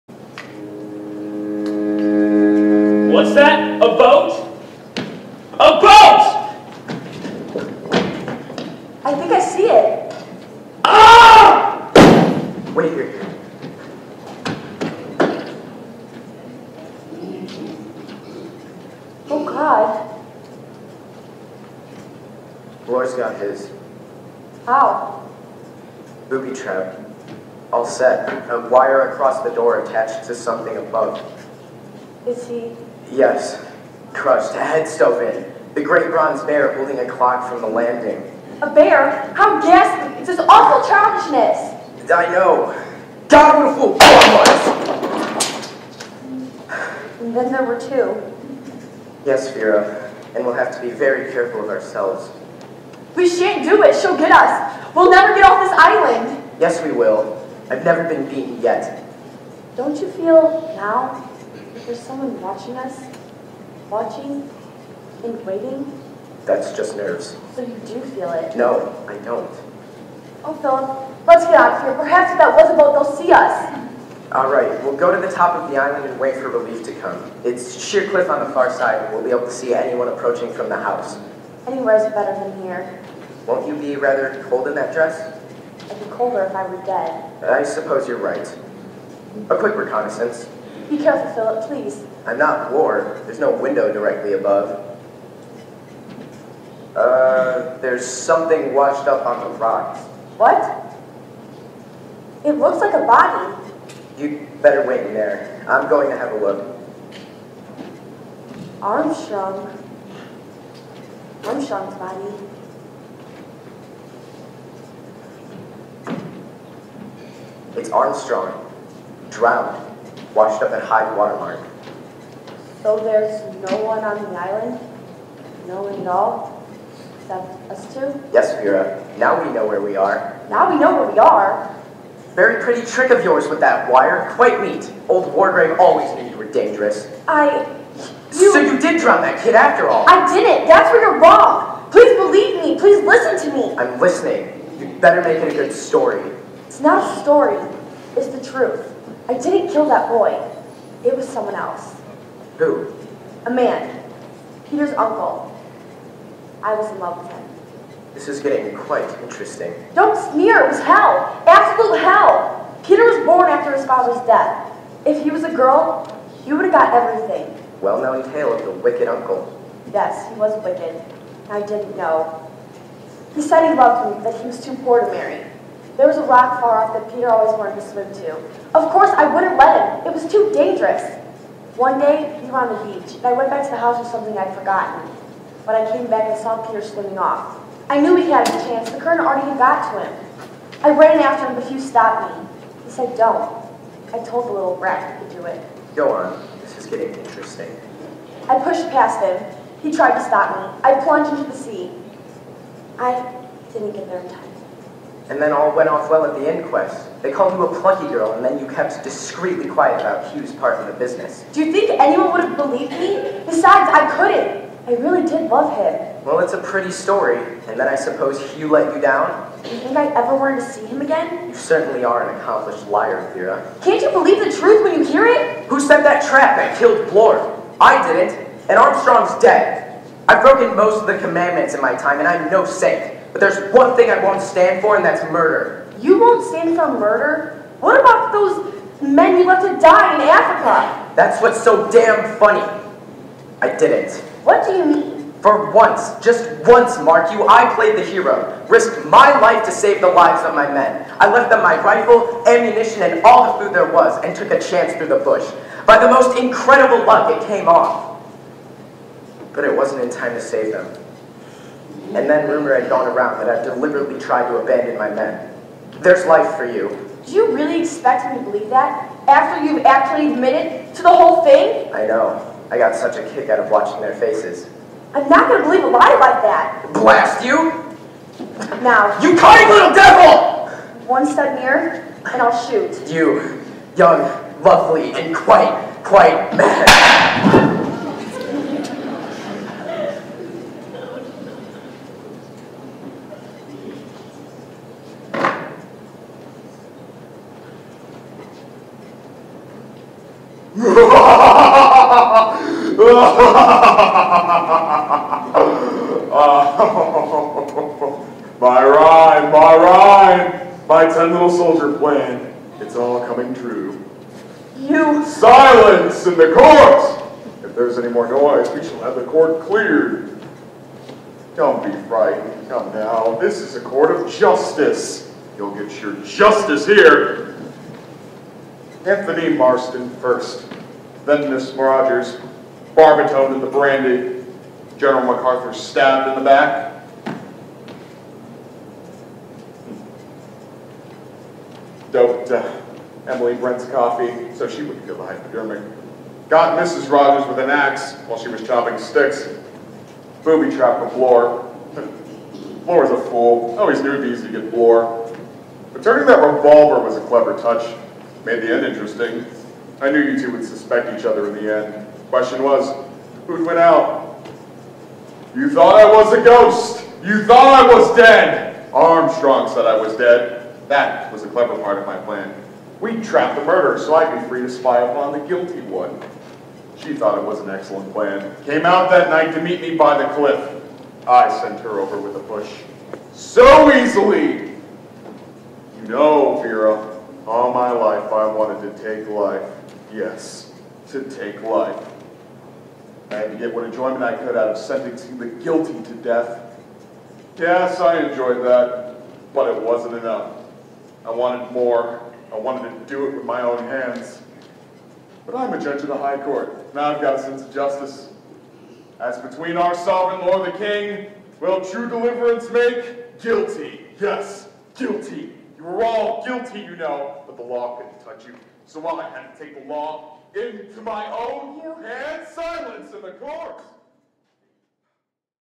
And Then There Were None Clip 5 Griffith Theatre Company Fall 2016
The Killer Revealed from "And Then There Were None" presented by The Griffith Theatre Company
Griffith High School Auditorium
601 North Wiggs Street
Griffith, IN 46319

Written By: Agatha Christie
Published By: Samuel French Incorporated

Produced & Directed By: Jenna Candiano
Assistant Director: Maggie Rivera
Set & Technical Director: Jim Graff
Costumer: Shannon Scheidel
Hair & Makeup Director: Laura Polise

The Cast:
Sir Lawrence Wargrave: Jesse Rosario
Vera Claythorne: Skylar Davenport
Philip Lombard: Cameron Bobos
Dr. Ellie Armstrong: Lara Reid
Mr. William Blore: John Vega
Miss Emily Brent: Sylvie Dowd
General MacArthur: Trevor Sheffield
Anthony Marston: Evan Queen
Mrs. Theresa Rogers: Nina Mendoza
Miss Ethel Rogers: Emma Loomis
Fred Narcott: Shawn Graham
The Voice: Madison Pearson

Student Production Staff:
Production Assistant: Maddie Nolan
Stage Managers: Corynn Kealey & Max Mclean
Makeup Director: Zack Keil
Hair Director: Christine Morikis
Costume Directors: Kiara Miller & Christina Dorsey

Facebook, Instagram, Tik Tok: Griffith Theatre Company

Mrs. Jenna Parker: Director